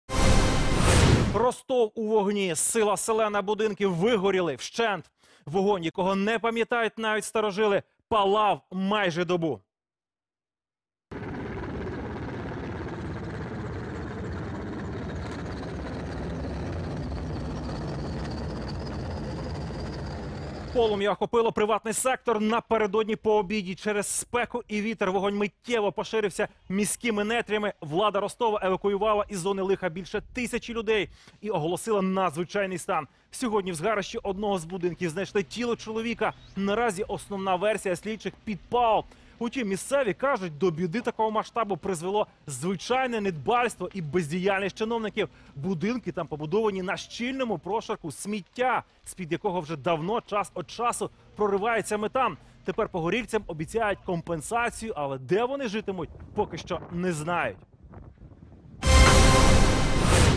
У Ростові вщент вигоріли майже всі будинки
Полум’я охопило приватний сектор напередодні по обіді.  Через спеку і вітер вогонь миттєво поширився міськими нетрями. Влада Ростова евакуювала із зони лиха більше тисячі людей і оголосила надзвичайний стан.